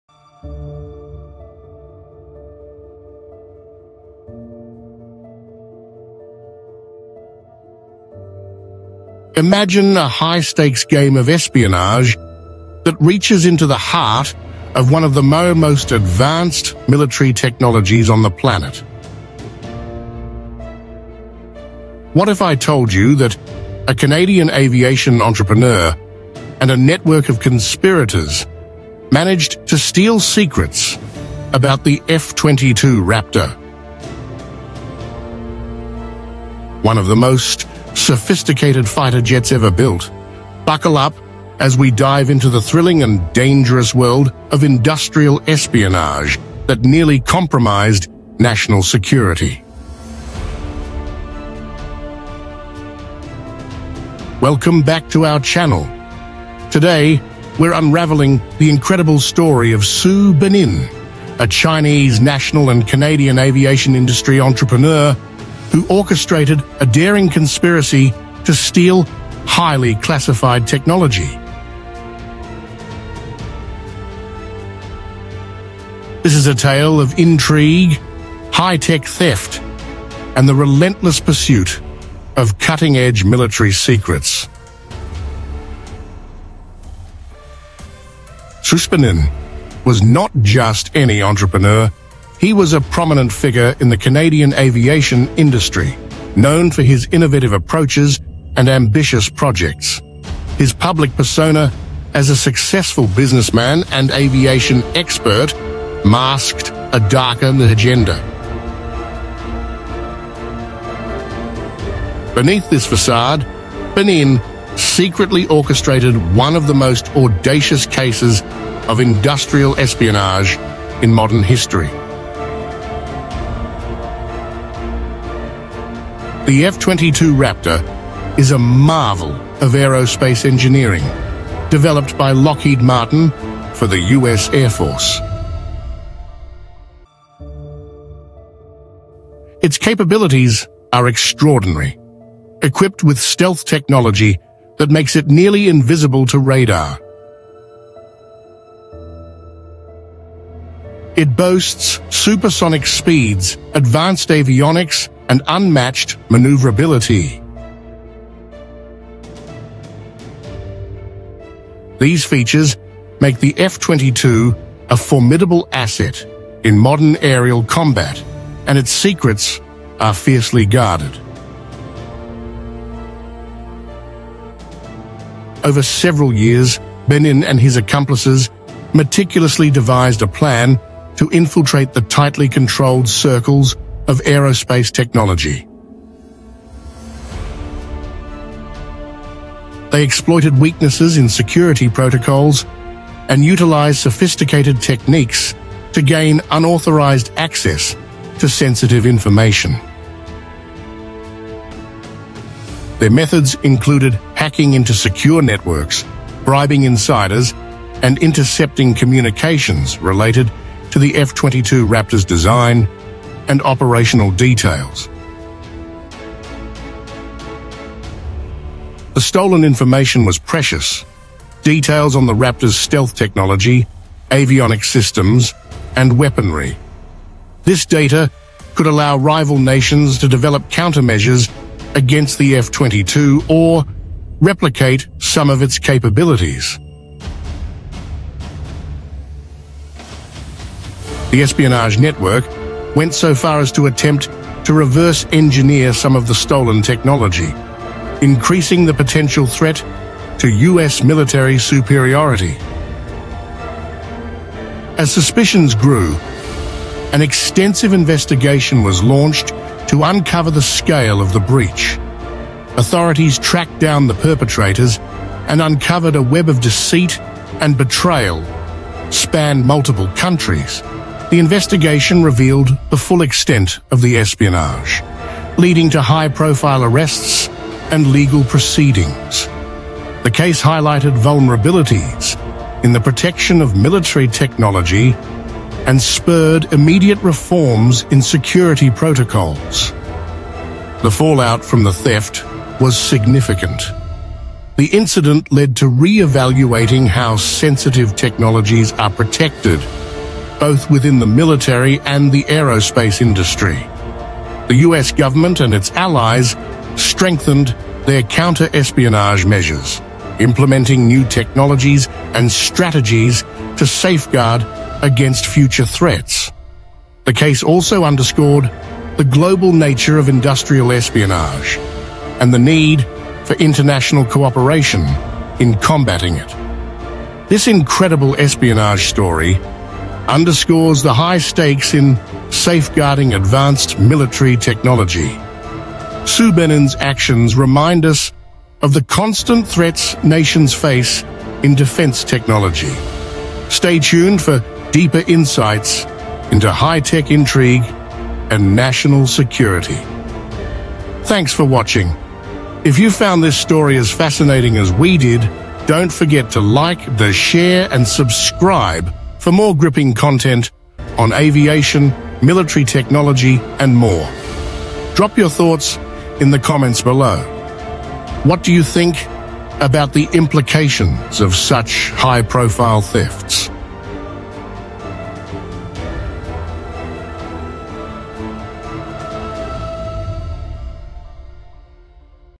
"The F-22 Raptor Theft "The Shocking Story the World Wasn't Ready For!"
Uncover the shocking F-22 Raptor theft scandal in this true crime story. Learn about the national security implications and aviation news surrounding this military scandal.
This gripping video delves deep into the shocking F-22 Raptor theft scandal that has left the aviation world in disbelief. Discover the intricate details behind one of the most audacious acts of espionage involving the world’s most advanced fighter jet. We explore how the theft occurred, the key players involved, and the implications for national security and military technology.

Join us as we unravel this incredible story, featuring exclusive interviews, expert analysis, and rare footage that sheds light on the events leading up to the scandal. Whether you're an aviation enthusiast, a military history buff, or just curious about high-stakes international intrigue, this video is a must-watch.